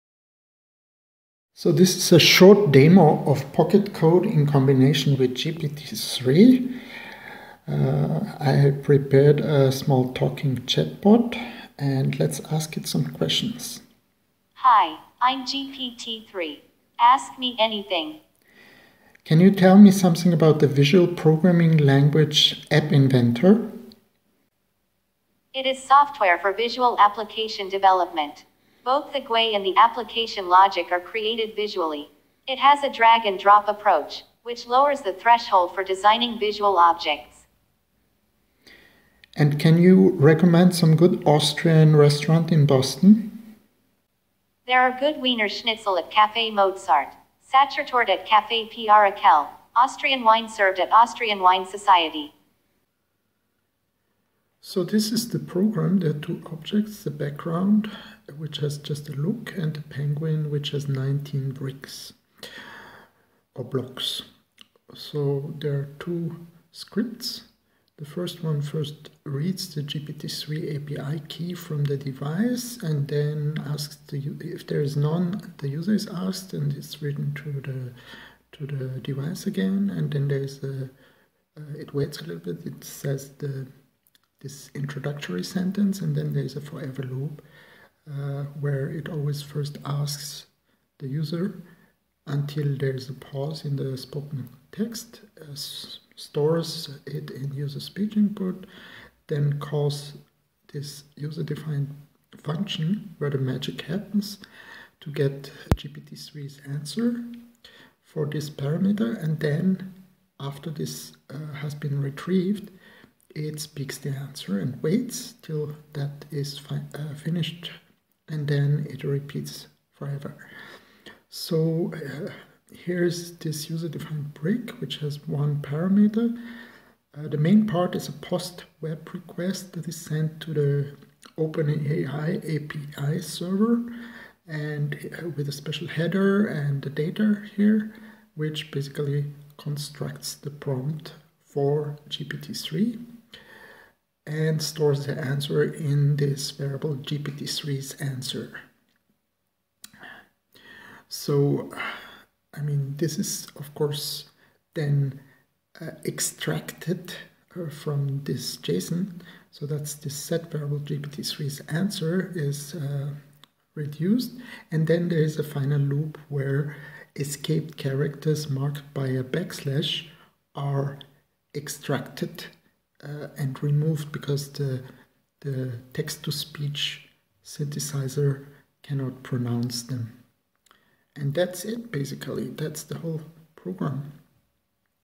Talking GPT-3 chatbot created with Catrobat's Pocket Code app
This was made with a pre-release version as of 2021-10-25 for a non-live demo for the MIT App Inventor team.